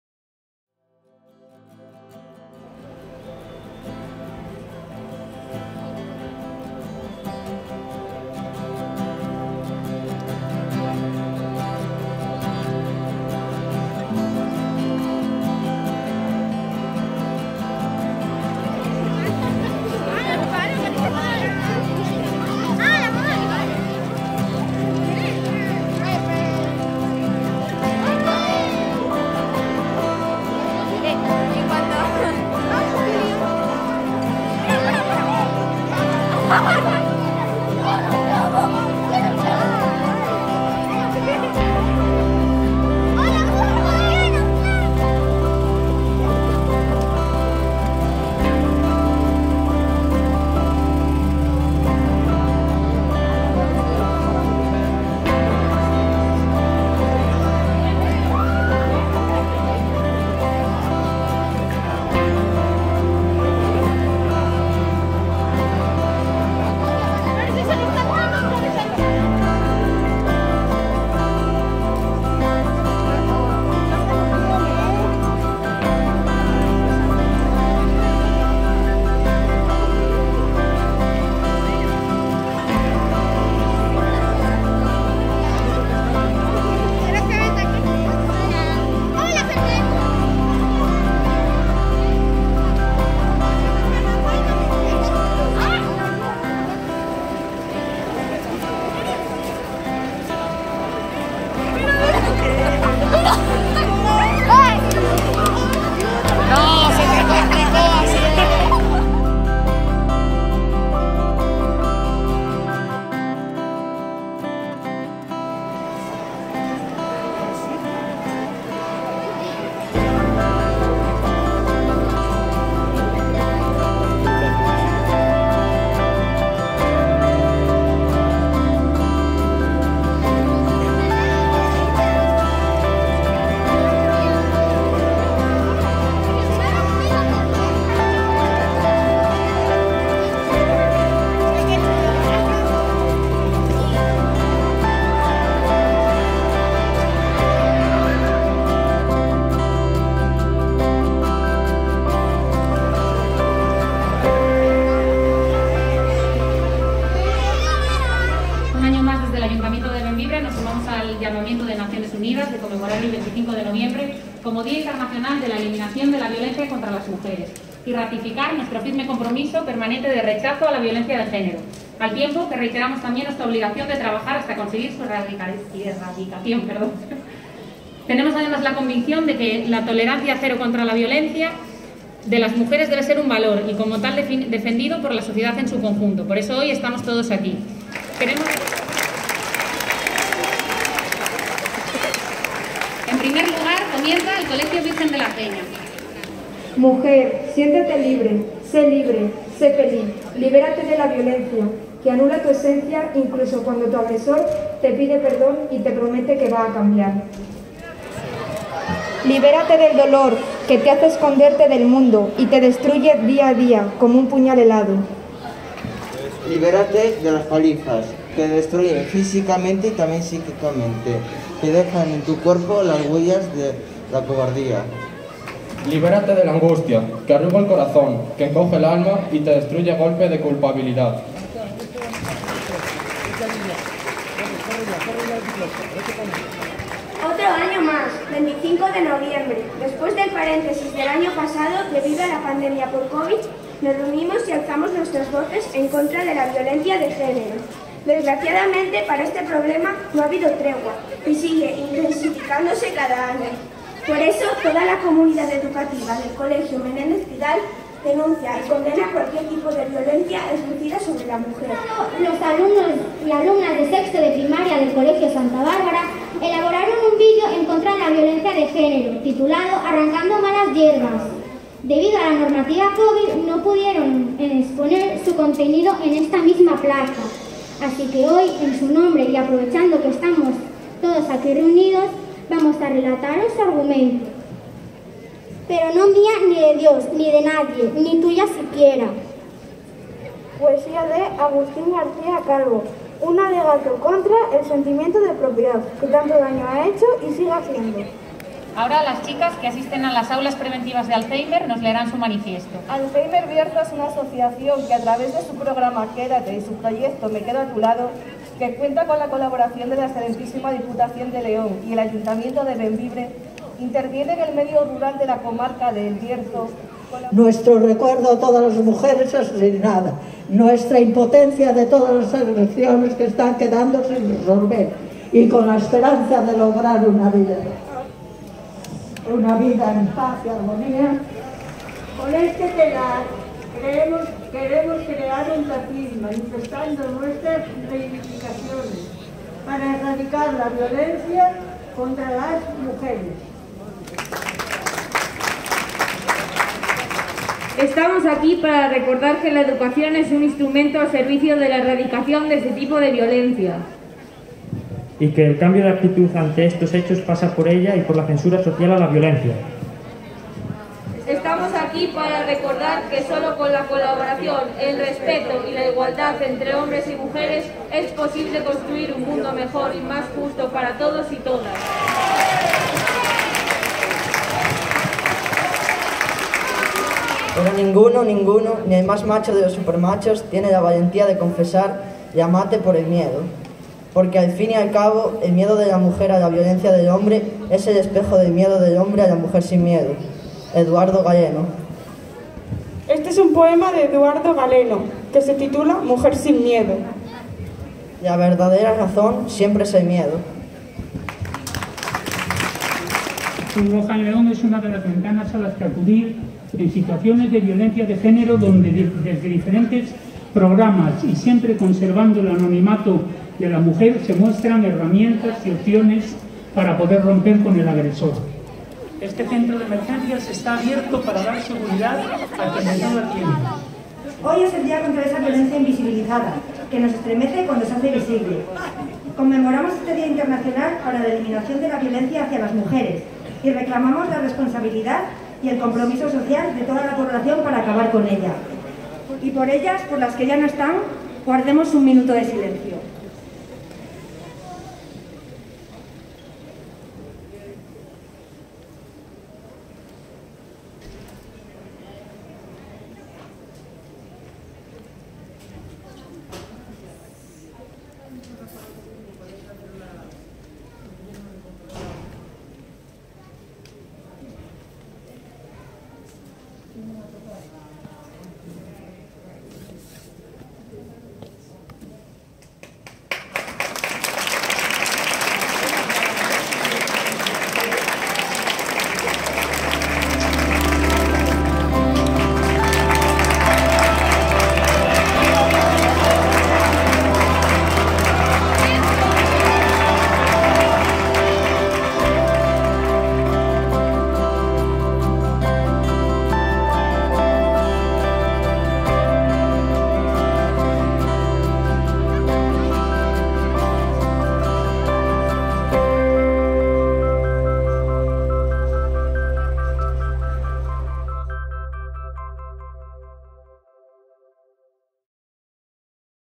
2021 Bembibre dice NO a la violencia de género
Las calles de Bembibre sirvieron este jueves para hacer ver las consignas frente a la violencia contra las mujeres.
Lo hizo con una marcha que partió desde la plaza Santa Bárbara, en la que participaron los colegios Virgen de la Peña, Menéndez Pidal y Santa Bárbara, y los IES Álvaro Yáñez y El Señor de Bembibre, alumnas de programas formativos de Alzhéimer Bierzo, los CEAS, Cruz Roja y una representación de la Corporación Municipal.